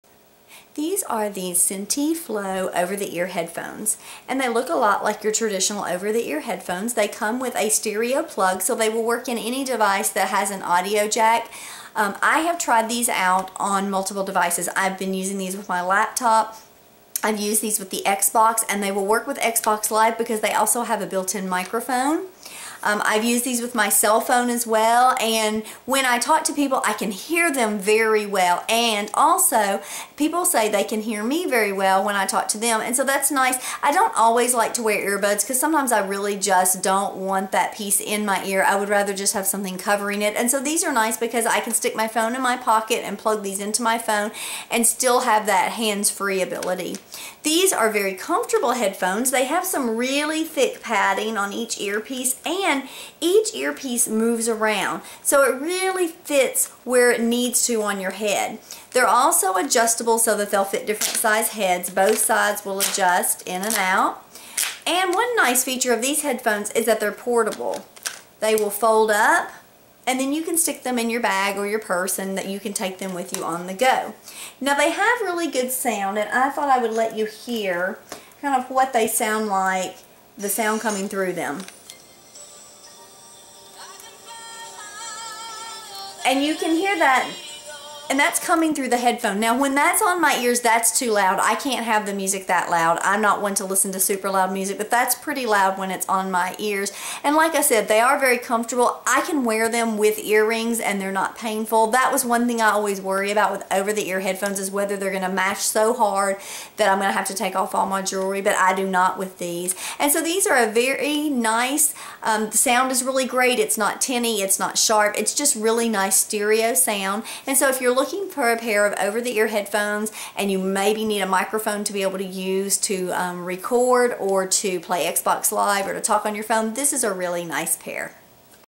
Sentey Flow On-Ear Headphones
These headphones have a great sound and a built in microphone.